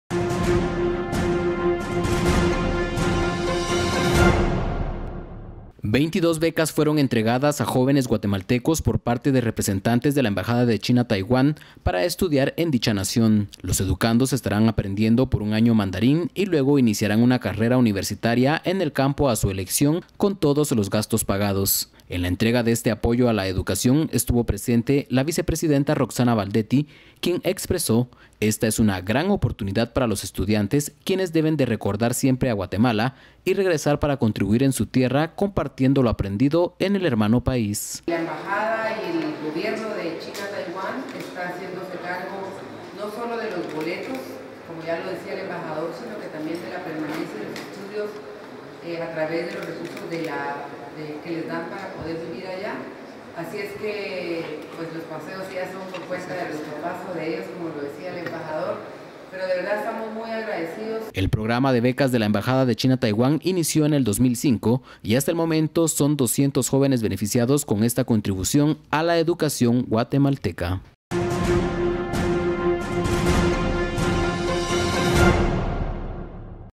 Vicepresidenta y embajador de Taiwán entregan becas
Por Jarbin Yelmo/DCA. China-Taiwán entregó a 22 jóvenes becas para que puedan cursar una carrera universitaria a su elección, en ese país asiático. Al acto asistió la vicepresidenta Roxana Baldetti, quien exhortó a los becarios para aplicar los conocimientos adquiridos a favor de Guatemala.